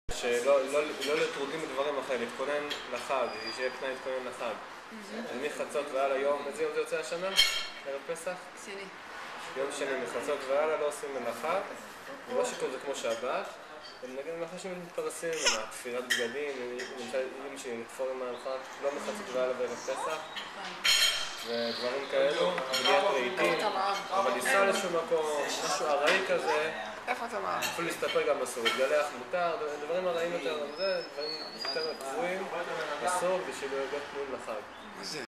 Tamar Playing at a Coffee Shop
In the background: divrey torah, courtesy of Raz.